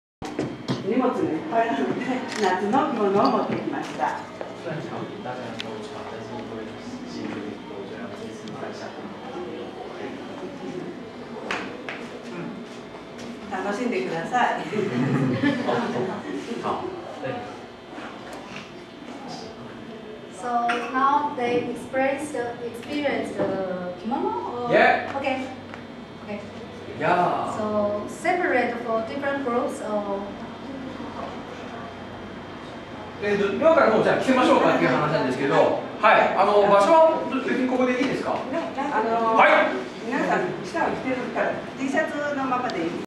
WISH CLUB in Taiwan / 台湾訪問 Part.02
2016年3月11日

台湾の内湖國民中学を訪問し、交流イベントとして着物体験と茶道体験を行いました。